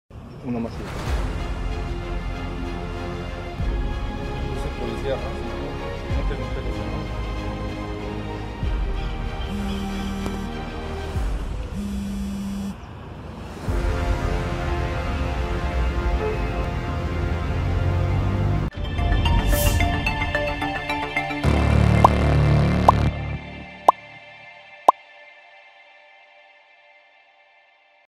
مسلحون يقتلون 5 أشخاص في محطة حافلات بالمكسيك
قتل 5 أشخاص وأصيب سادس برصاص مسلحين هاجموا، الاثنين، محطة للحافلات في كويرنافاكا، المدينة السياحية في جنوب المكسيك، على ما أعلنت السلطات.